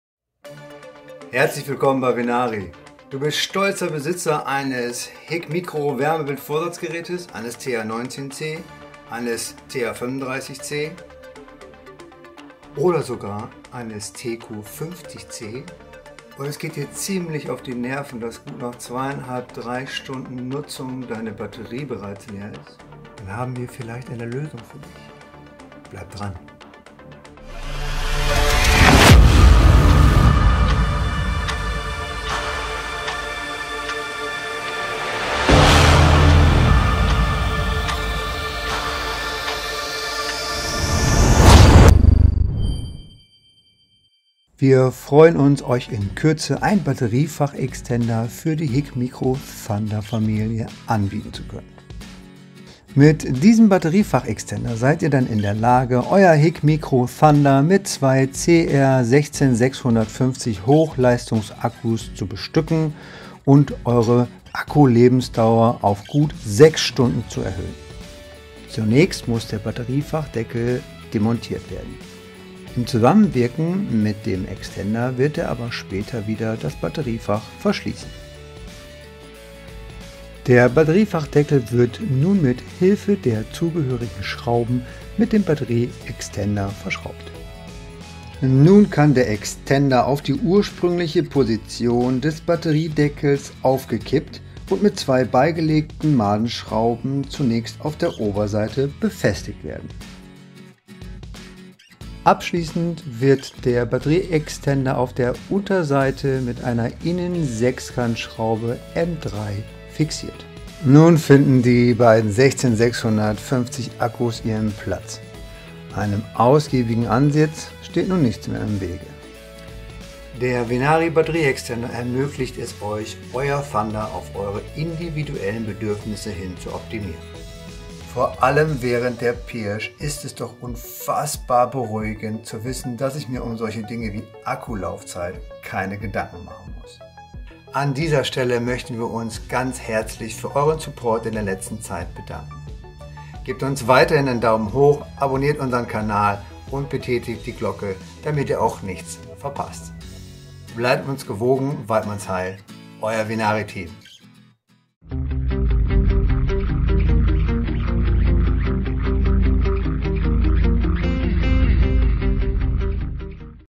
Акумуляторний кейс HIKmicro Thunder для акумуляторів великої ємності. Muraveinyk
Акумуляторний кейс HIKmicro Thunder для акумуляторів великої ємності

Розширення батарейного відсіку для використання батарей
16650. Значно збільшений термін служби батареї HIKmicro Thunder.
Не потрібний зовнішній блок живлення або заміна батареї.

VENARI Power Extender – це подовжувач батарейного відсіку серії HIKmicro Thunder. Цей корпус, який можна прикріпити збоку, подовжує відсік батарей, дозволяючи вставляти великі батареї 16650, що значно продовжує термін служби насадки і робить непотрібною заміну батарей.

Інструменти, необхідні для корпусу батареї VENARI Thunder:

Імбус 0,9 мм
Шестигранний ключ 2,5 мм
Хрестова викрутка типу «ph00/2мм»